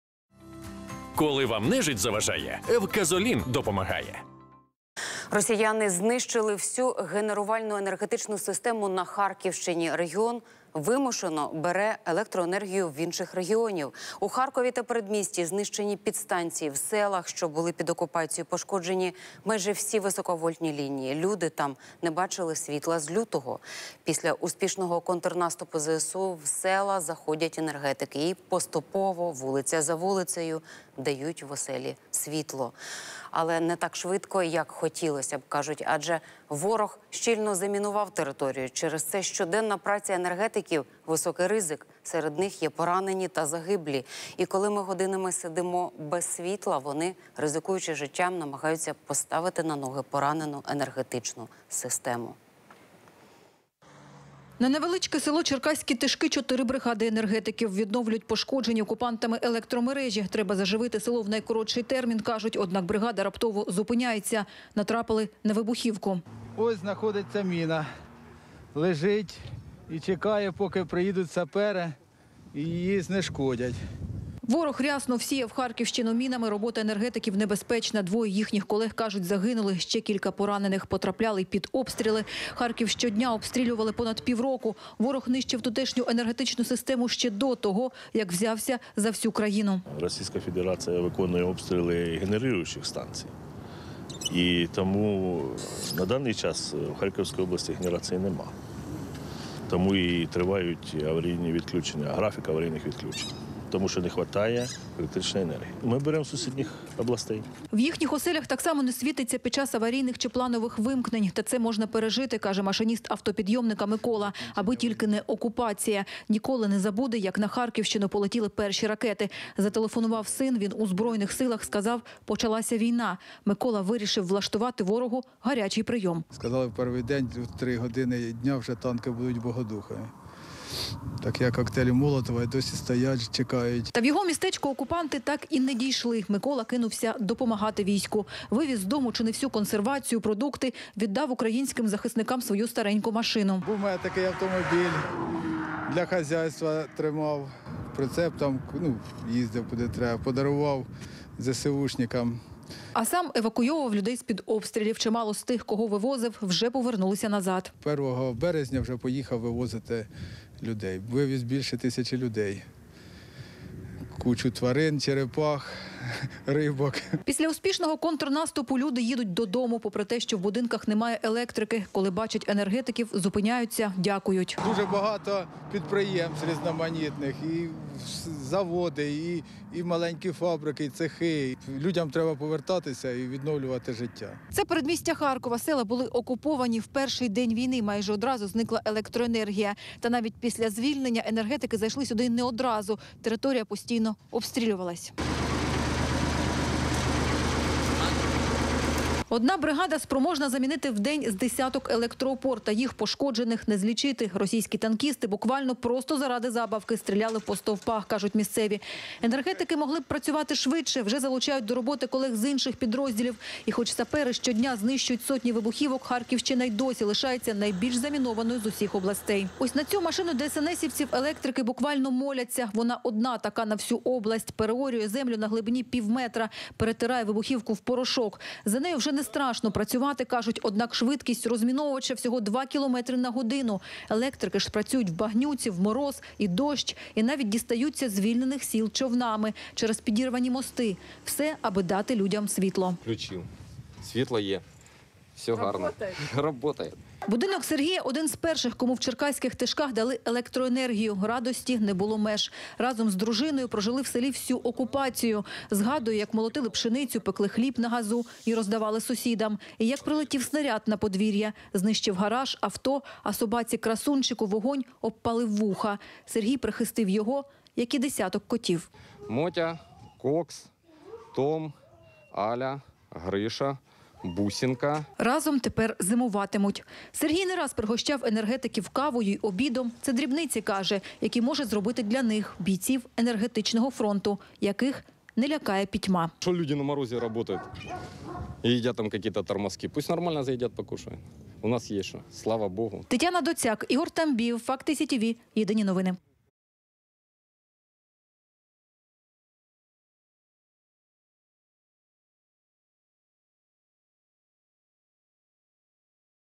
На Харківщині лагодять енергетичну систему СЕРЕД МІН
Росіяни знищили всю генерувальну енергетичну систему на Харківщині, регіон вимушено бере електроенергію в інших регіонів.

У Харкові та передмісті знищені підстанції, в селах, що були під окупацією, пошкоджені майже всі високовольтні лінії — люди там не бачили світла з лютого.

Після успішного контрнаступу ЗСУ, в села заходять енергетики, і поступово, вулиця за вулицею, дають в оселі світло.

Але не так швидко, як хотілося б, кажуть, адже ворог щільно замінував територію, через це щоденна праця енергетиків - високий ризик, серед них є поранені та загиблі.

І коли ми годинами сидимо без світла, вони, ризикуючи життям, намагаються поставити на ноги поранену енергетичну систему.